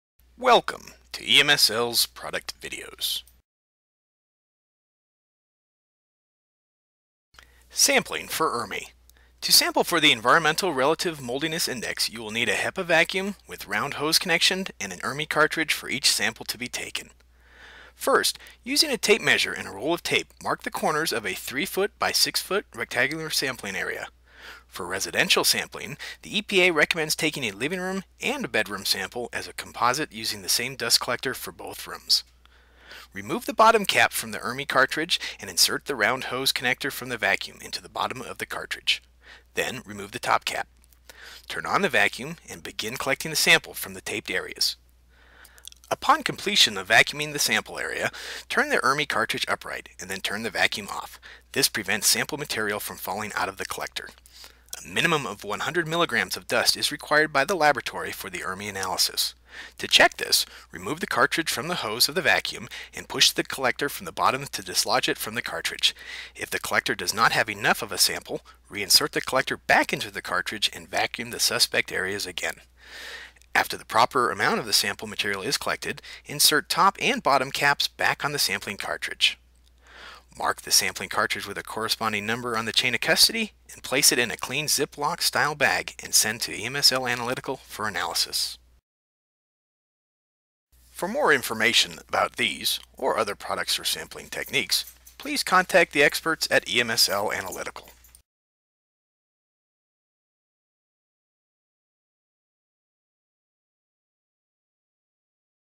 ERMI Sampling Guide from EMSL Analytical
Guide to sampling for the Environmental Relative Moldiness Index (ERMI)